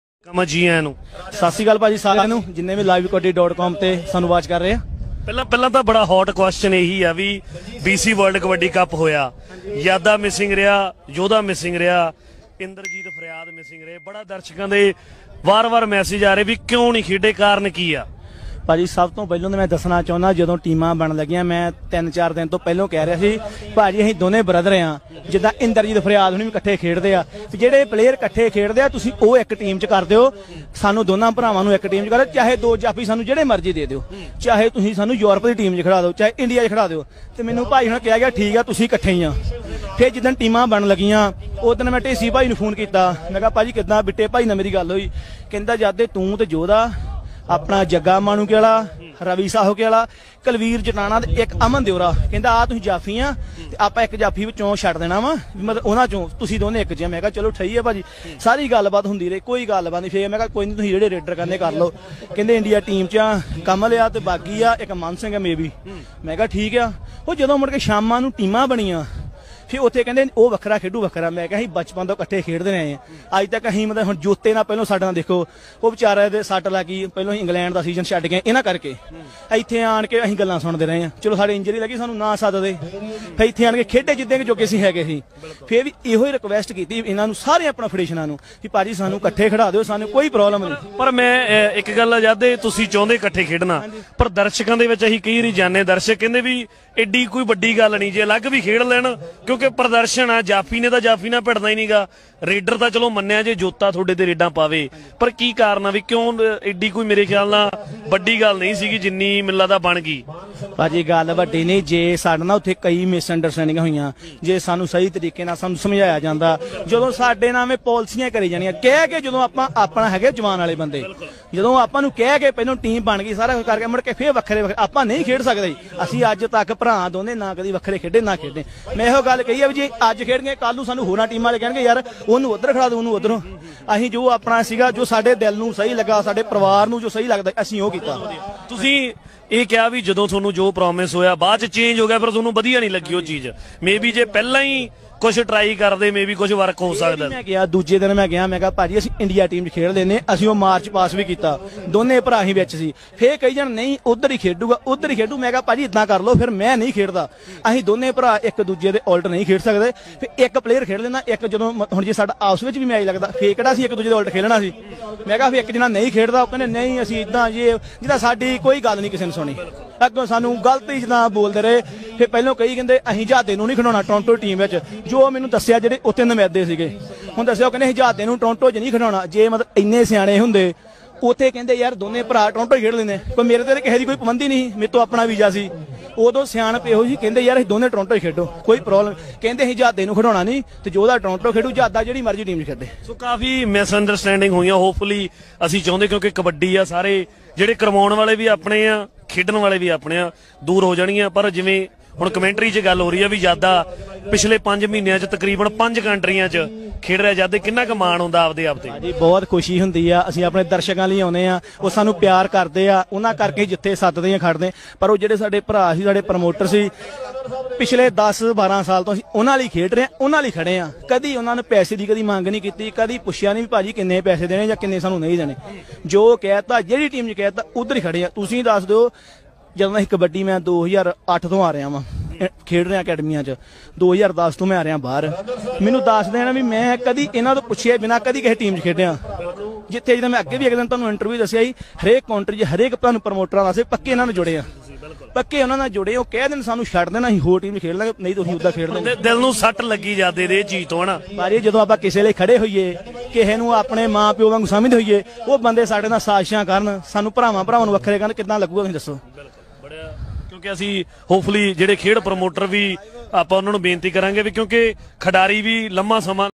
i😡 Yadha Surkhpur ਨੂੰ ਆਇਆ Kabaddi Promoters ਤੇ ਗੁੱਸਾ, ਕਹਿੰਦਾ ਸਾਨੂੰ ਦੋਨਾਂ ਭਰਾਵਾਂ ਨੂੰ ਇਹ ਅਲੱਗ ਕਰਦੇ ਆ
Sign in